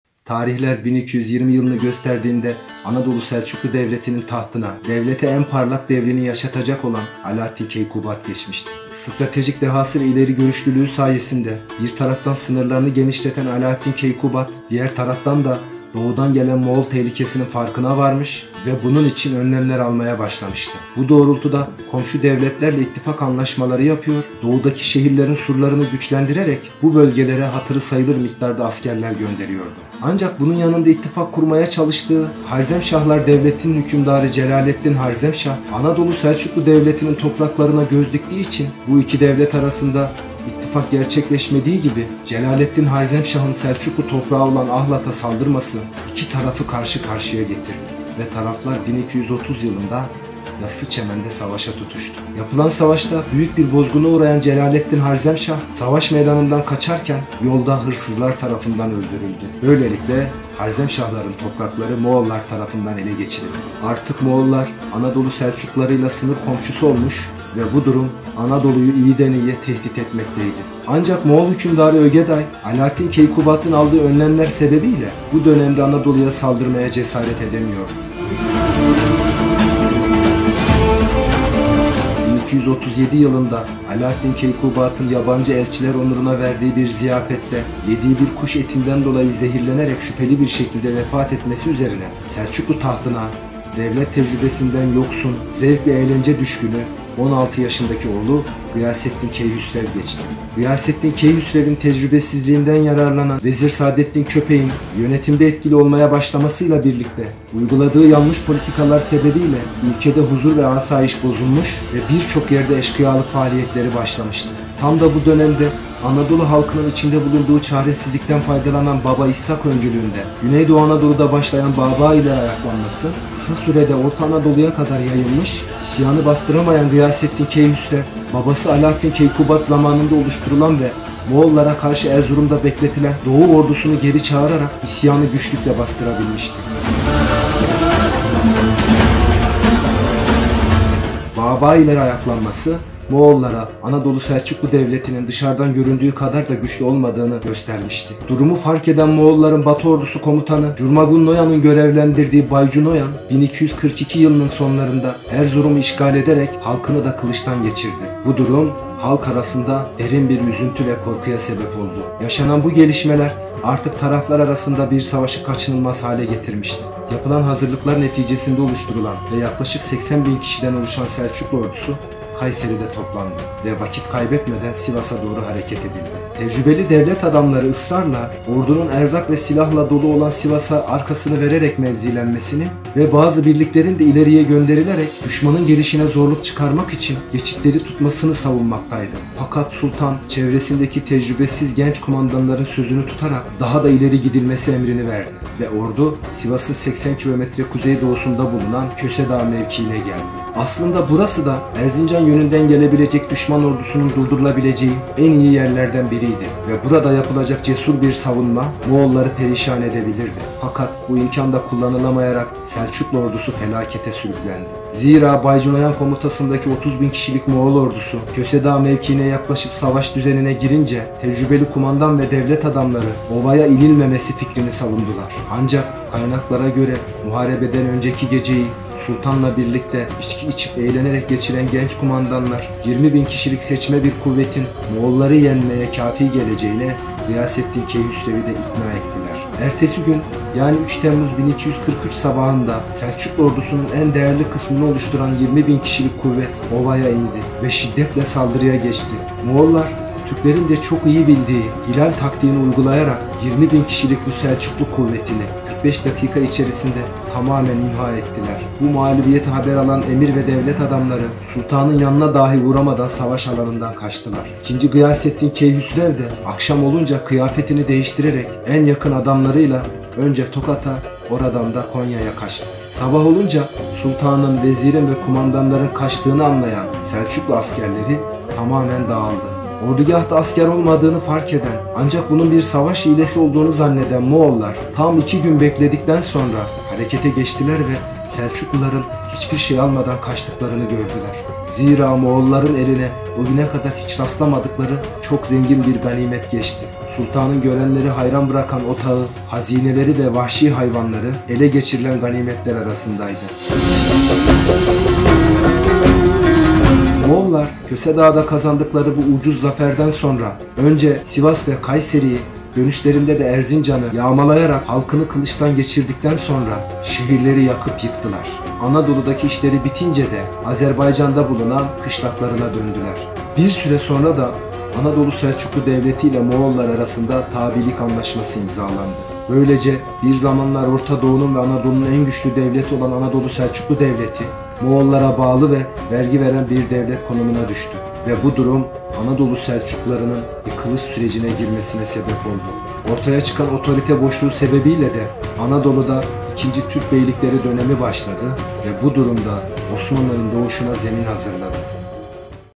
Kösedağ Savaşı ve Sonuçları
Bu videoda Anadolu Selçuklu Devleti ile Moğollar arasında gerçekleşen Kösedağ Savaşı'nı en çarpıcı yönleriyle anlatmaya çalıştım.